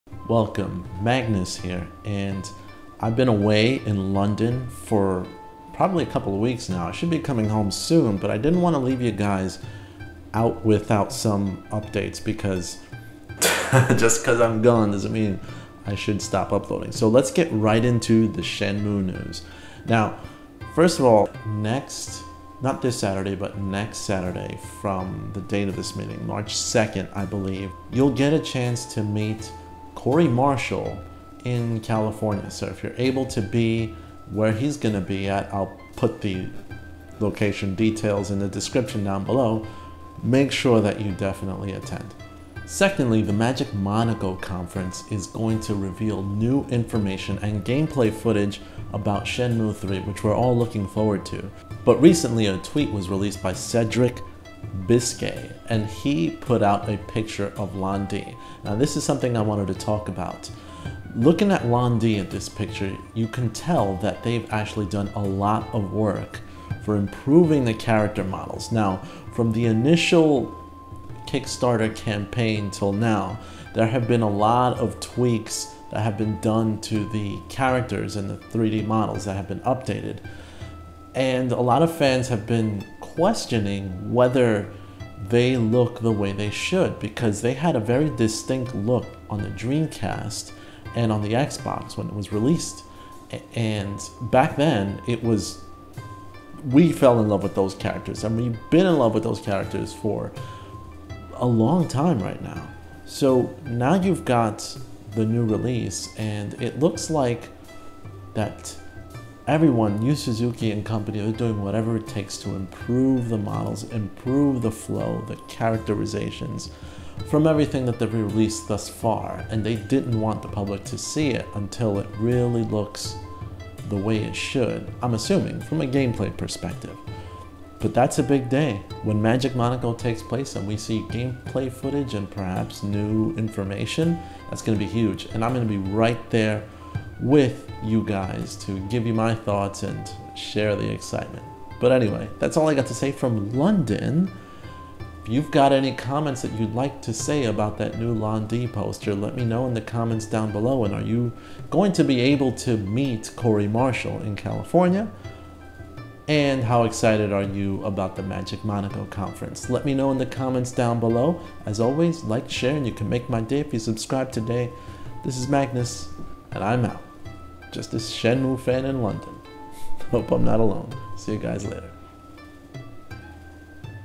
Shenmue 3 News update - What the new pictures say about the game.
It's been a while since my last post, but I've been travelling!  I'll be back shortly, but until then, check out my update and comments on the new Shenmue pics that were announced on Twitter.  Also I share my thoughts on the progress of the game thus far.

If you'd like to meet Corey Marshall, he will be at Game Realms in Burbank, CA on March 2nd, 2019.